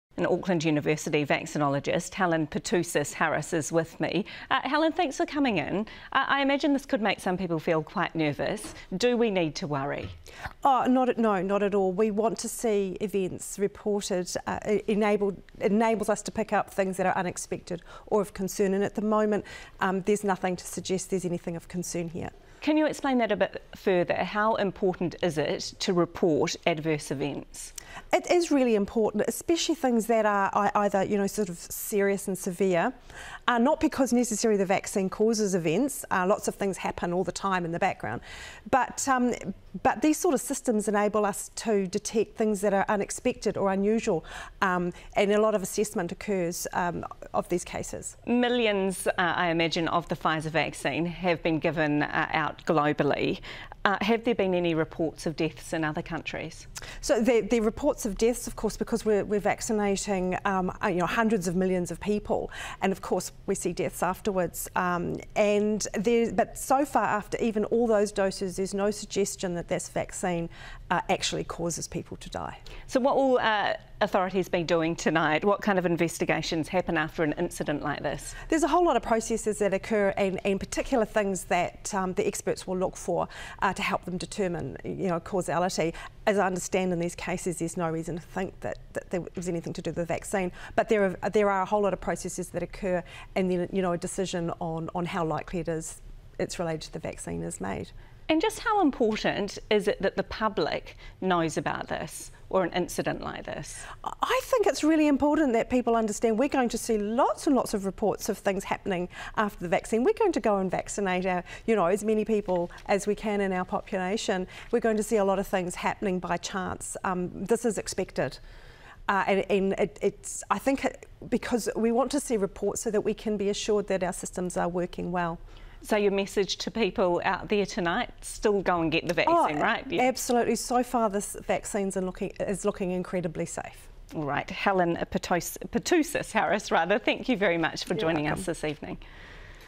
Deaths of people who had Covid vaccination ‘no cause for concern’ – vaccinologist
The deaths of two people who died after receiving the Covid-19 vaccination are under investigation, but Dr Helen Petousis-Harris told 1 NEWS was no reason for people to worry.